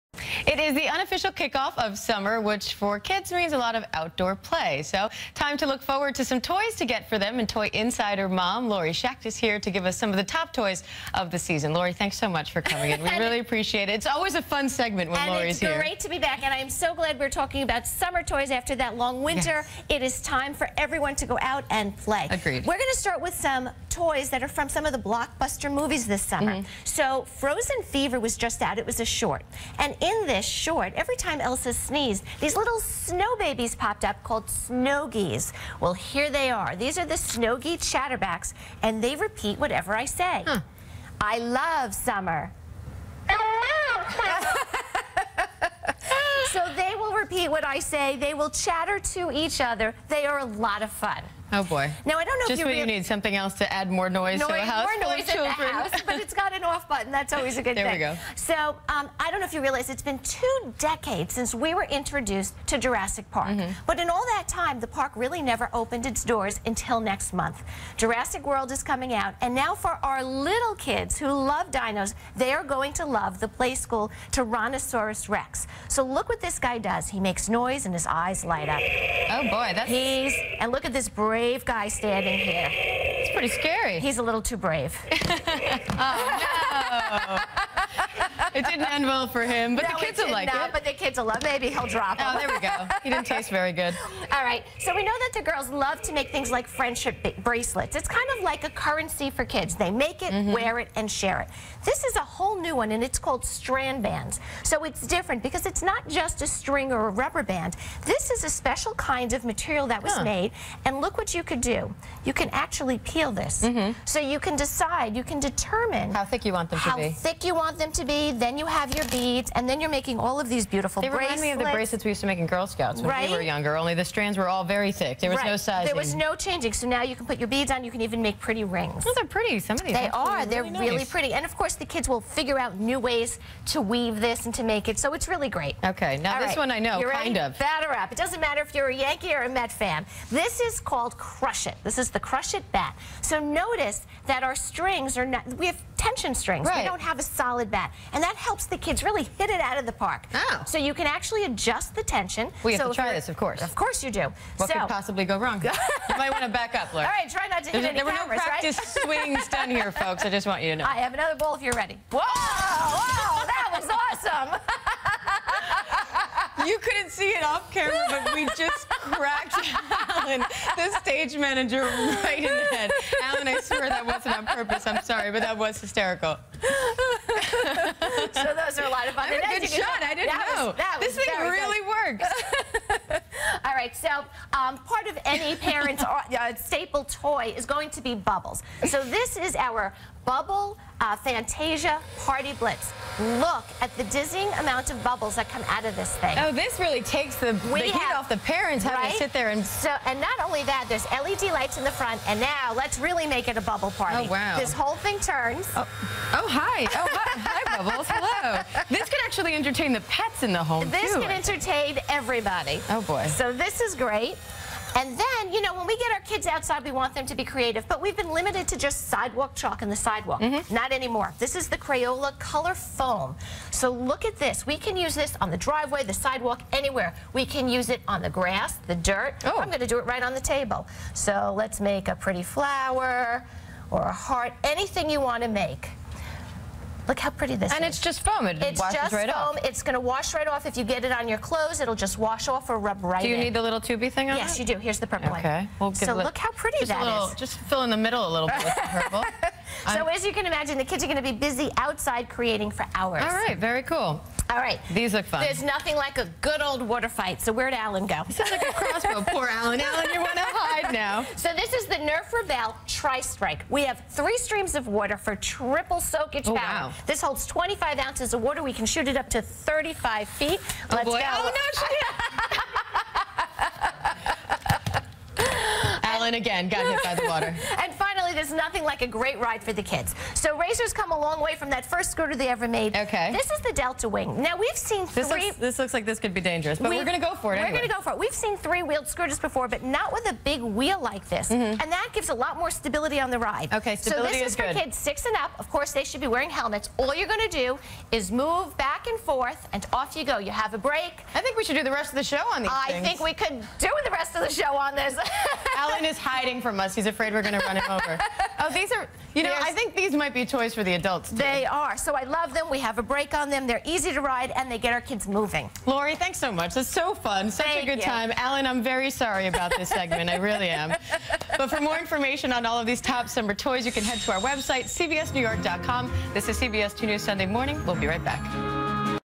Hot Summer Toys
&nbsp;Toy Insider Mom Laurie Schacht stops by the CBS2 studios to discuss with Diane Macedo some of the hot toys this summer.